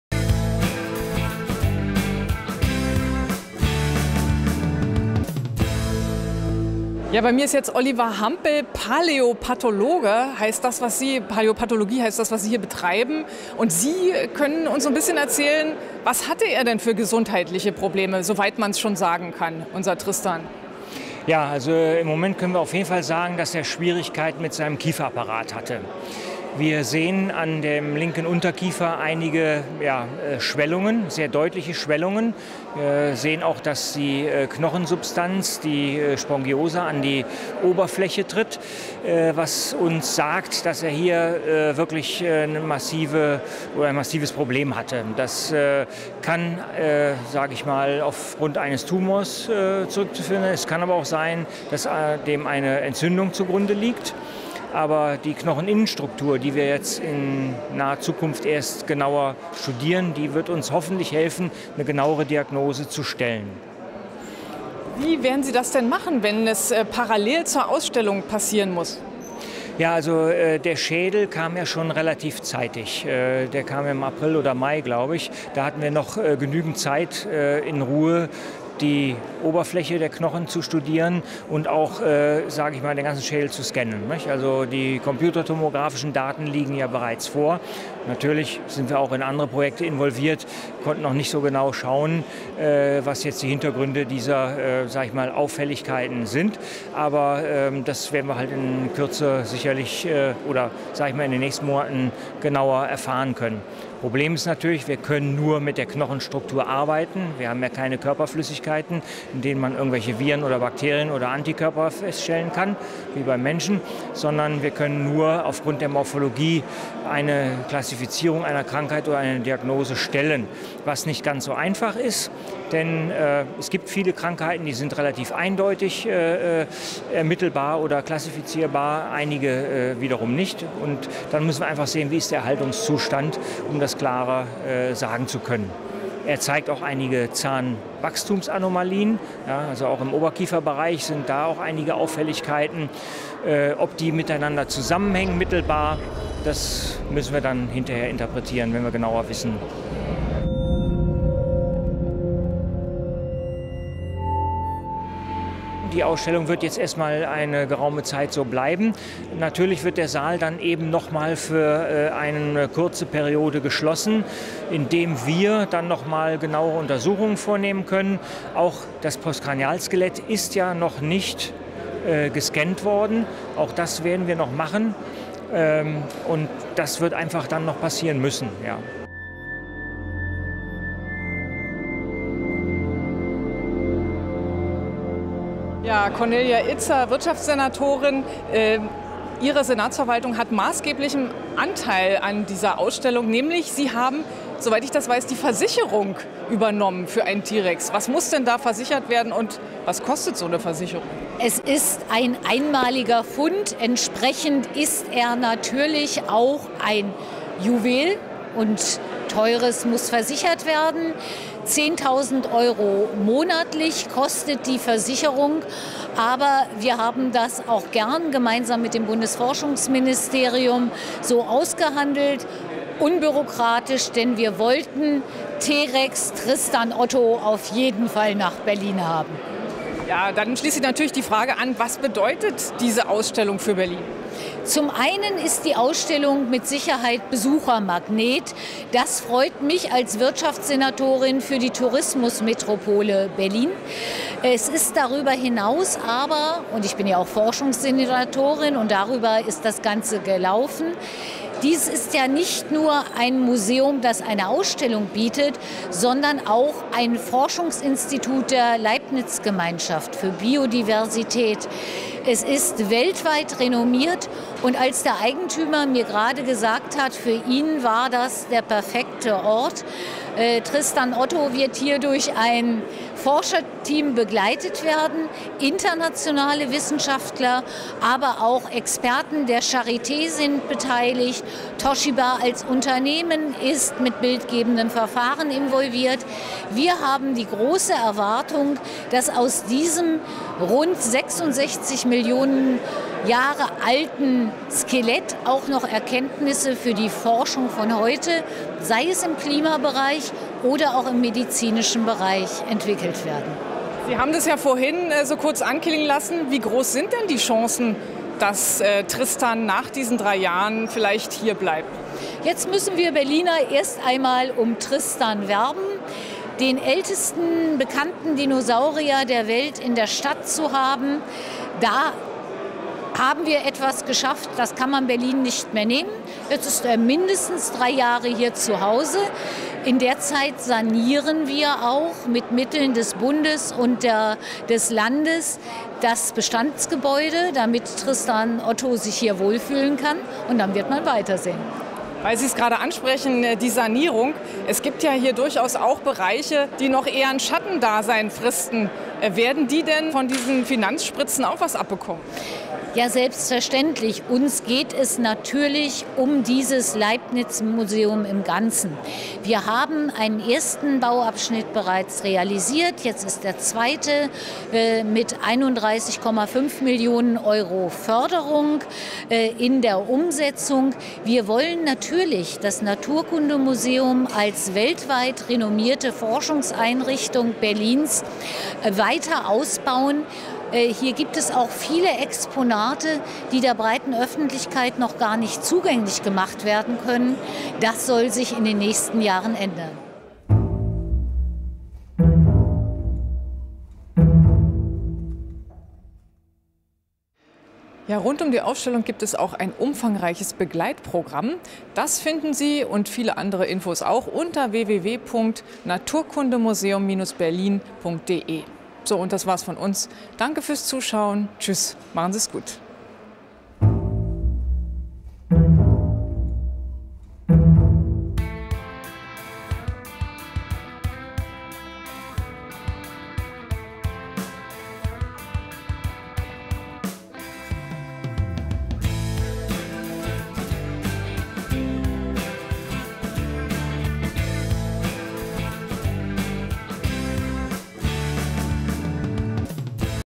Sehenswert! // T-rex im Berliner Naturkundemuseum (Teil 3)
Berlin ist um einen Publikumsmagneten und um ein einmaliges Forschungsobjekt reicher: mit Tristan ist  eines der weltweit am besten erhaltenen Exemplare eines Tyrannosaurus rex in einer eigenen Ausstellung zu besichtigen. Das zwölf Meter lange Skelett und der nahezu vollständige Schädel des gigantischen Raubsauriers sind zu großen Teilen im Original zu sehen und derzeit einmalig in Europa.